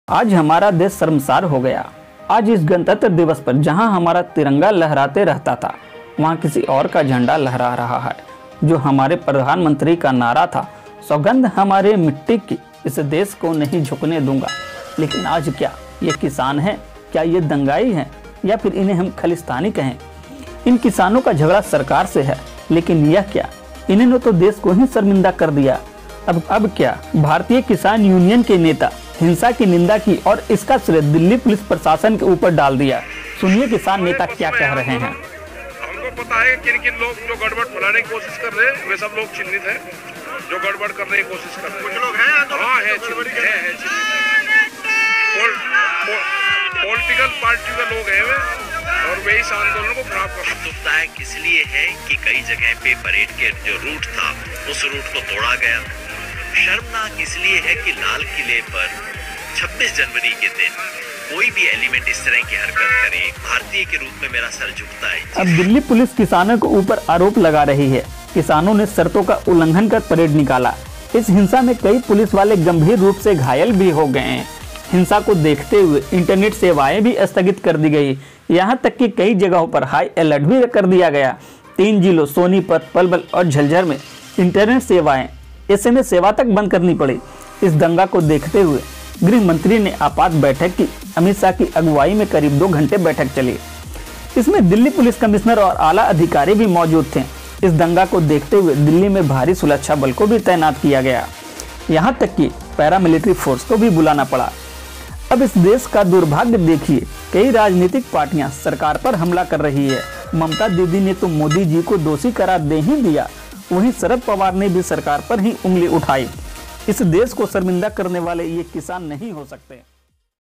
ट्रैक्टर रैली के बाद किसान आंदोलन ! सरकार एक्शन में है ! Farmar Protest ! Kishan Andolan
ट्रैक्टर रैली के बाद किसान आंदोलन ! सरकार एक्शन में है ! Farmar Protest ! Kishan Andolan Kuch Fact

farmer protest Kisan aandolan, Gazipur border, tractor ke bad Kisan andolan, farmer protest, किसान आंदोलन farmer protest aandolan